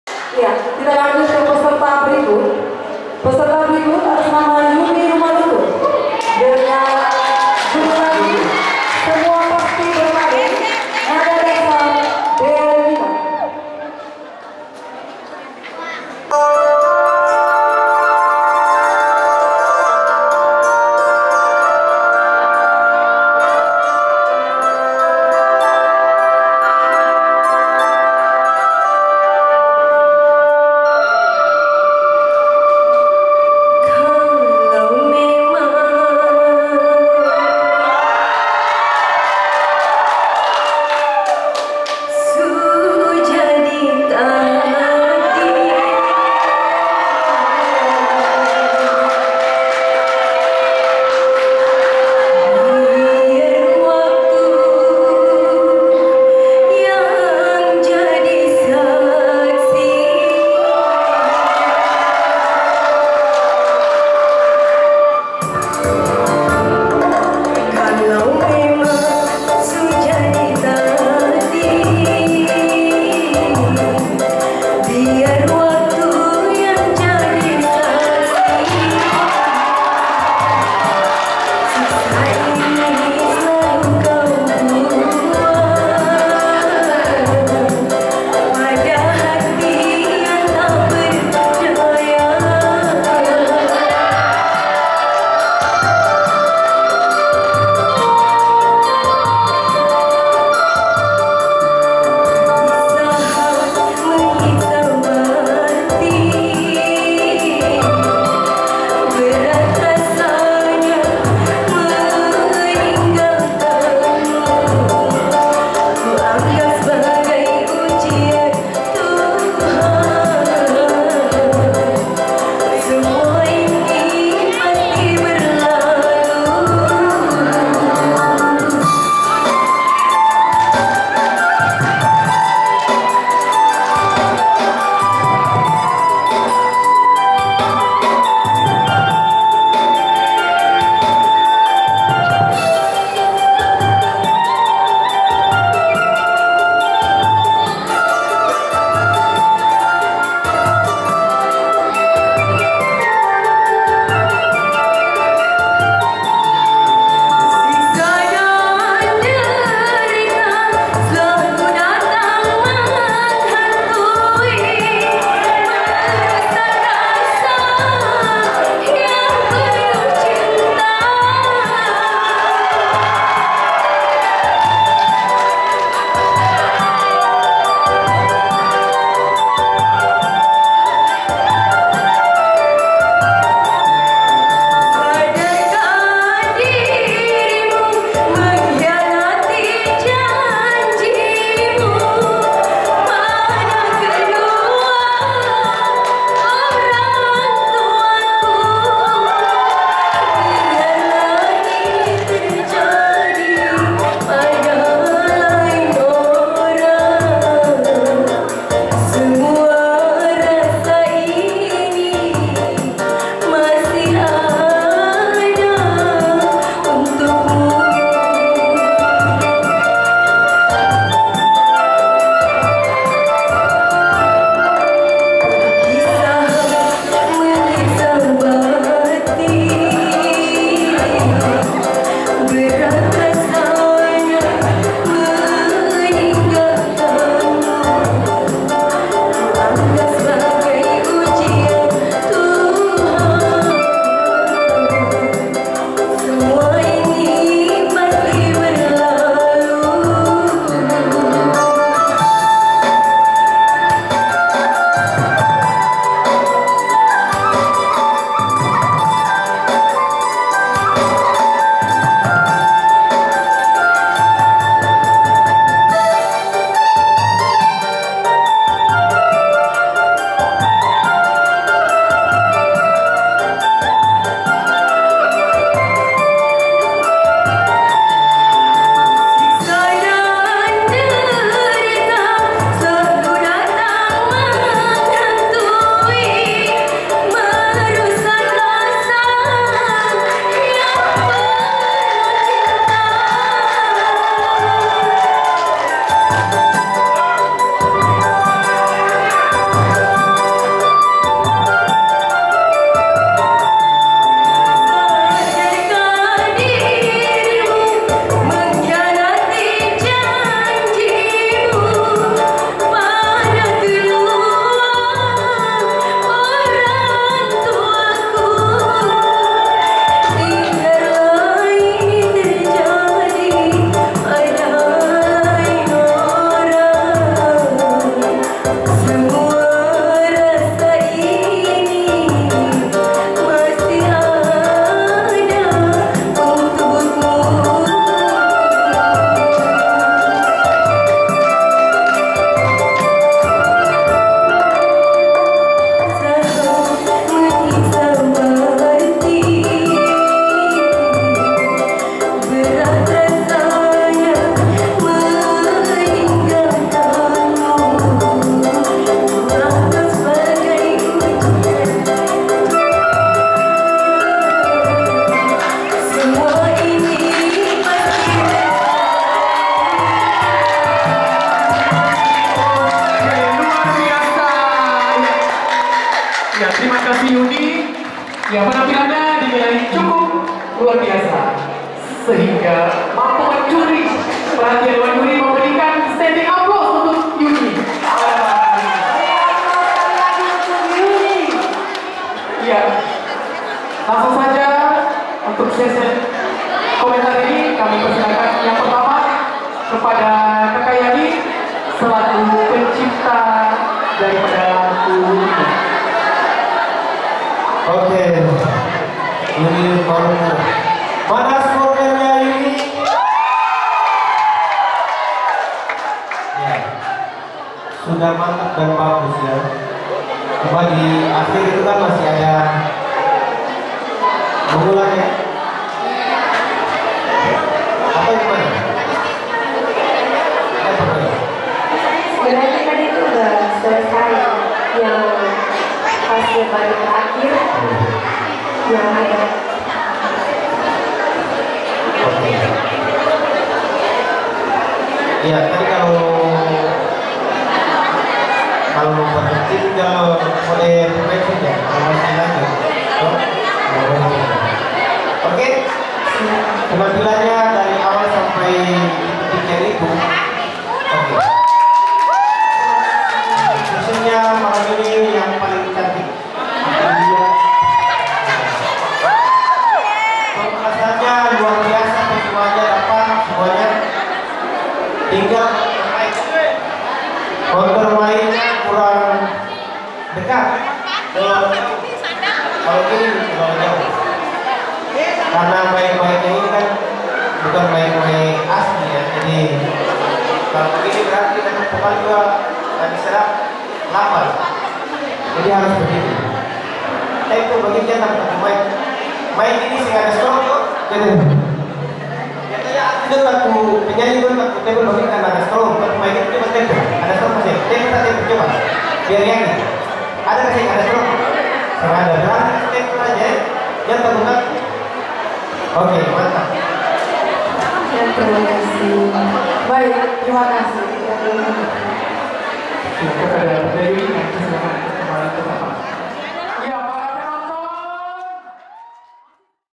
Walau Sakit, Yuni Mampu Kontrol Vocal di Malam Final DANGDUT SINGING CONTEST NIR 1 MASOHI 2021
Malam pembukaan semi final Dangdut Singing Contest Nusa Ina Record 1 Masohi Tahun 2021